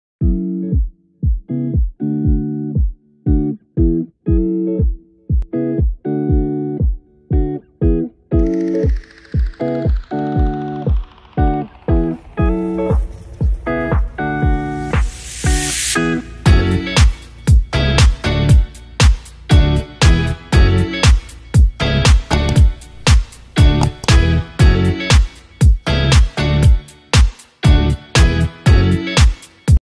Dwarf Peach Tree / Peach picking
This is the dwarf peach tree we have in the backyard and luckily have one sweet fruit this year.